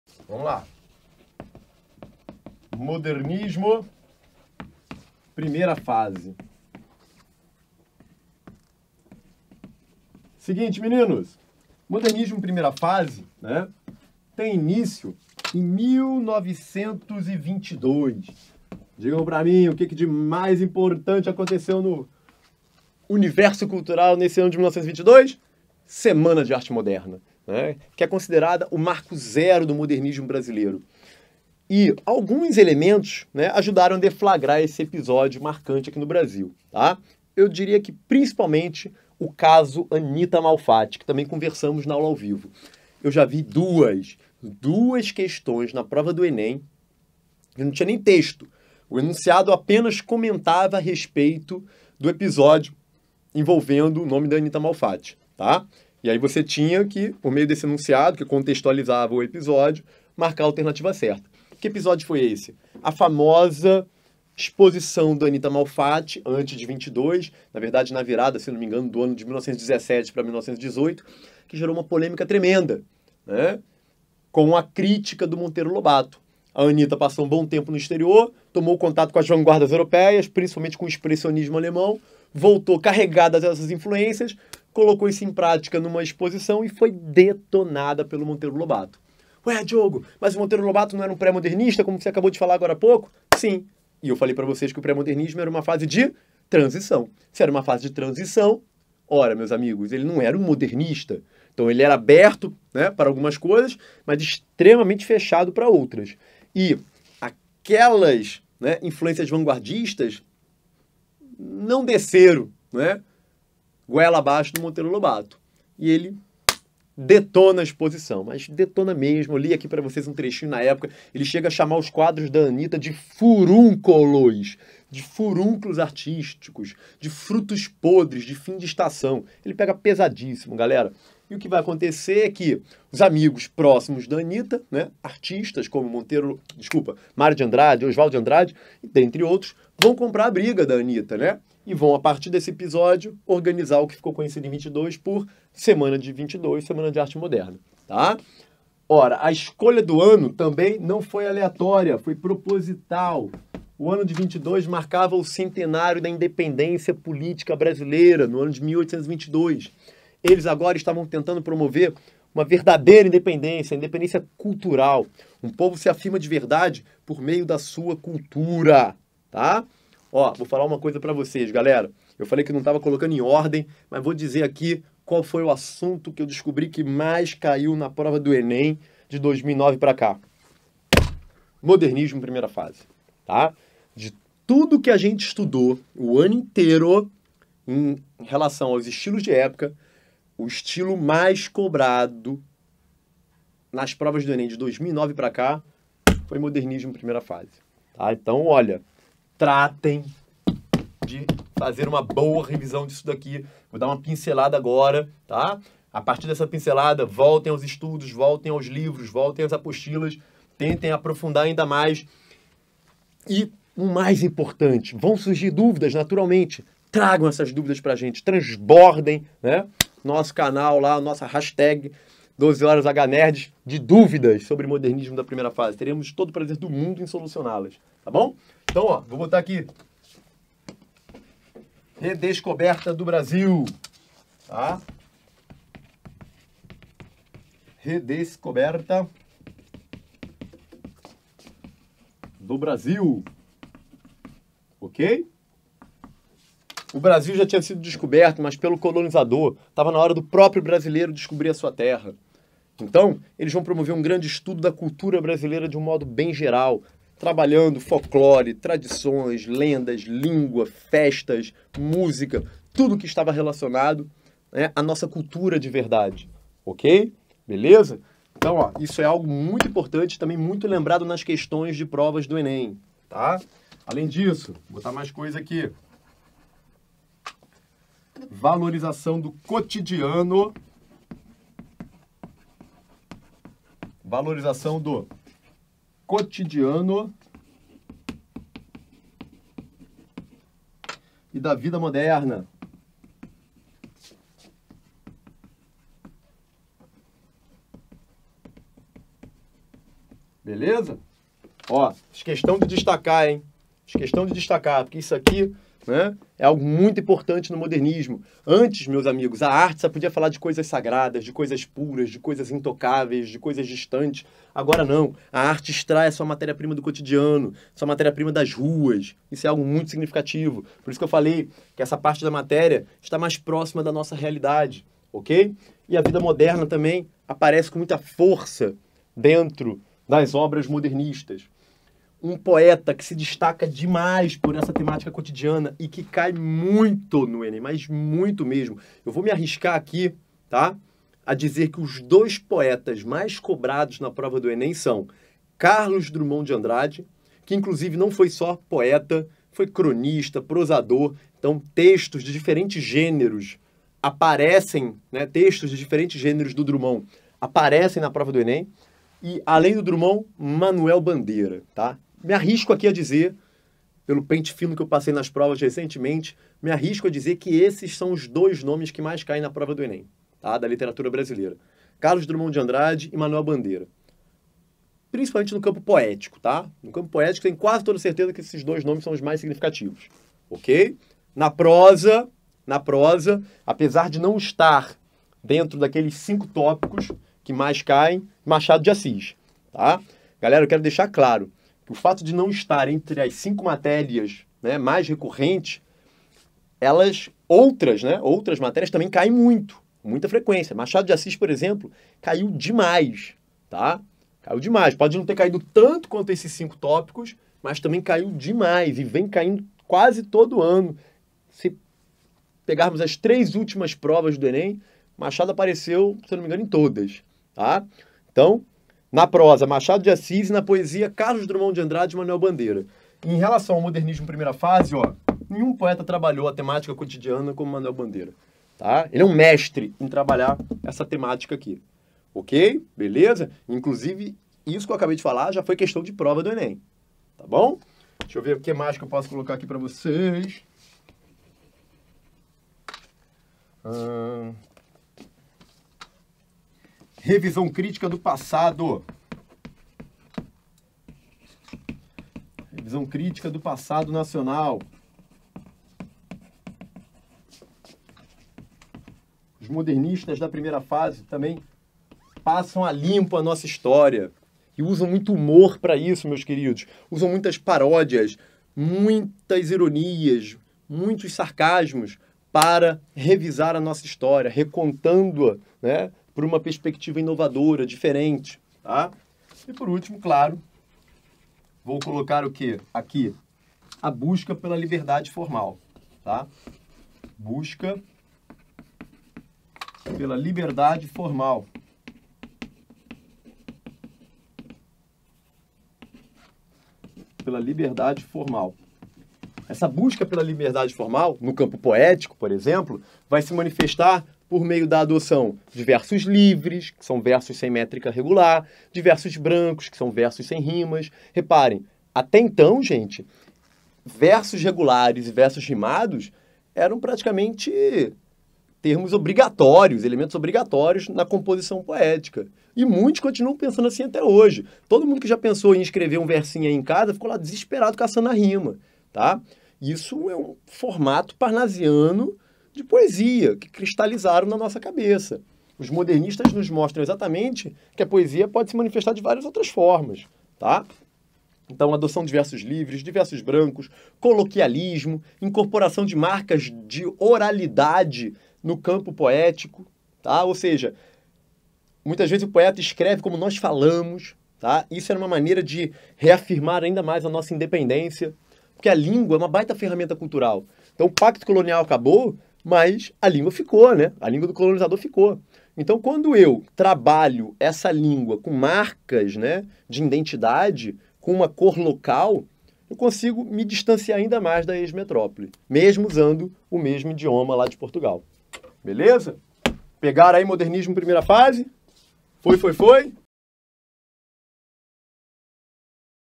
Revisão ENEM - Literatura: Primeira Fase do Modernismo - Mande dúvidas com
No mês do ENEM os Fadões do Descomplica estão revisando a matéria e tirando todas as dúvidas do Brasil! Mande as suas com 12hNerds para chegar ao ENEM totalmente pronto